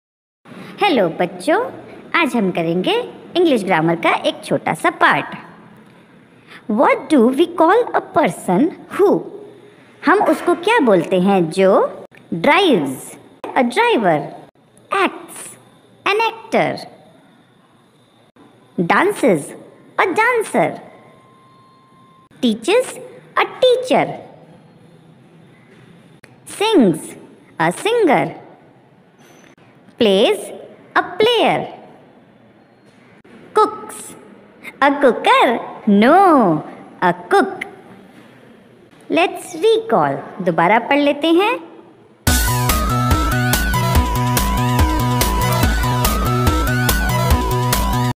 What do we call a person whoजो ये करता है हम उसे क्या कहेंगे,जो आदमी cook करता है उसे cooker कहेंगे?
English Grammar